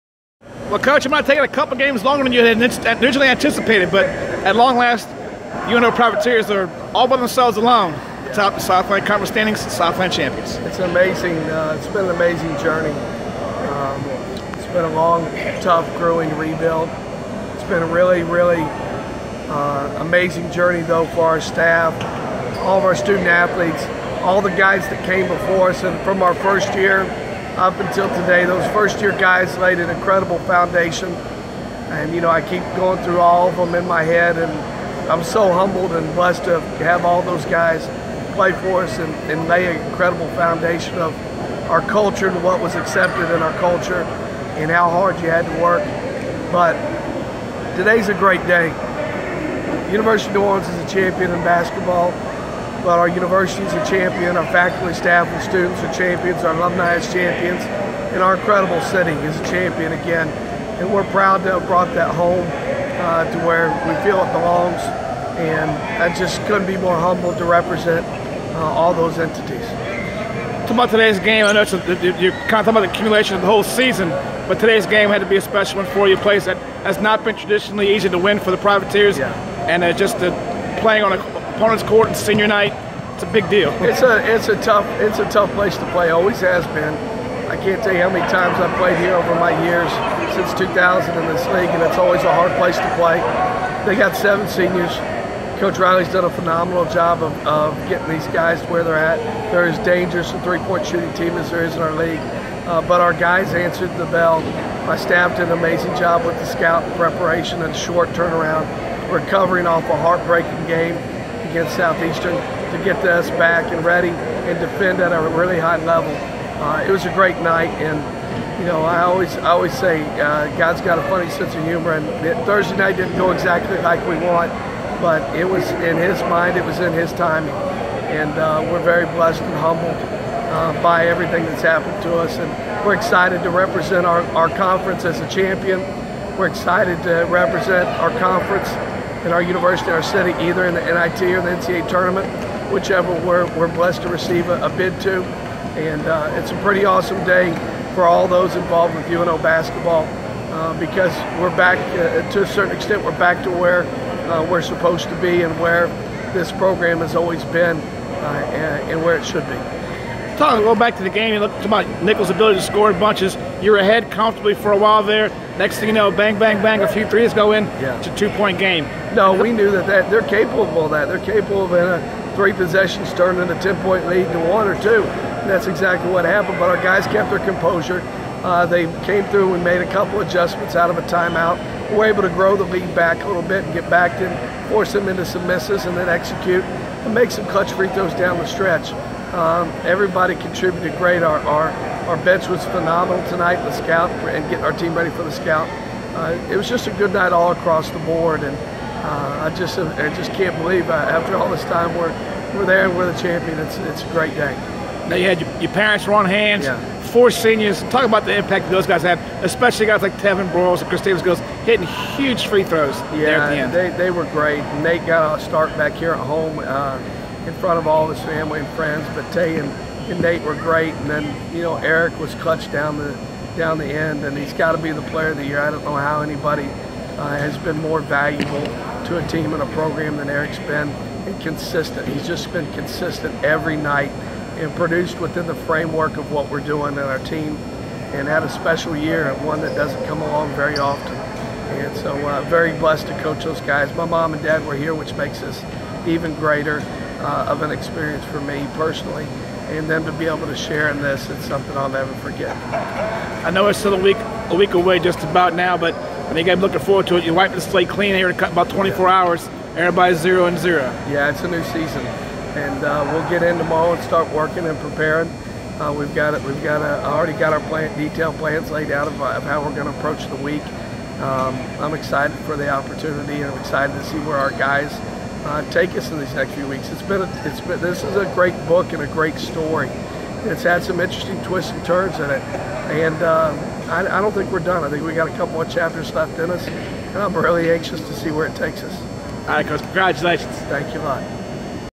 Coach Slessinger Reviews UNO's 74-64 Win At Nicholls (03/04/17)
University of New Orleans head men's basketball coach Mark Slessinger reviews the Privateers' 74-64 Southland Conference win at Nicholls on March 4, 2017. With the victory, UNO claims sole possession of the 2016-17 SLC Championship, the No. 1 seed in the upcoming conference tournament and guarantees itself a spot in the postseason.